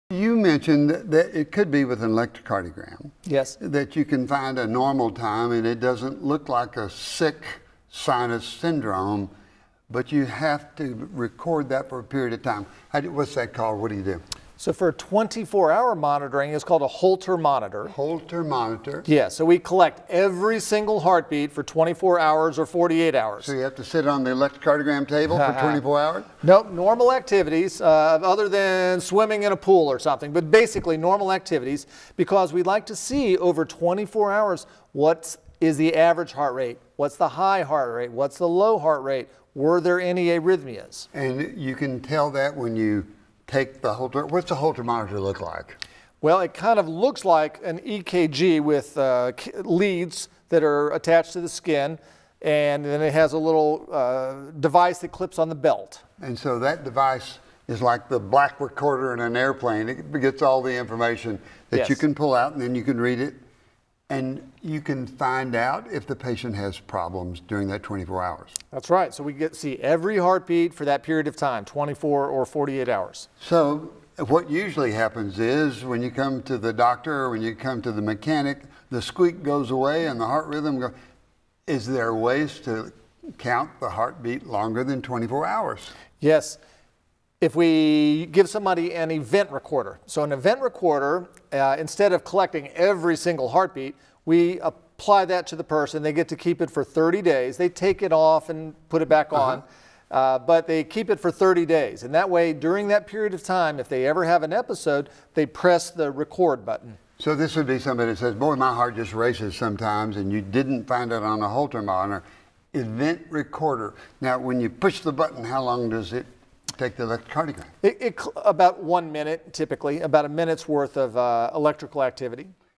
How to Record Your Heart Rate
Dr. Malcolm Foster, Board Certified Cardiologist, explains two different ways a specialist can record your heart rate over an extended period to help diagnose issues related to the heart.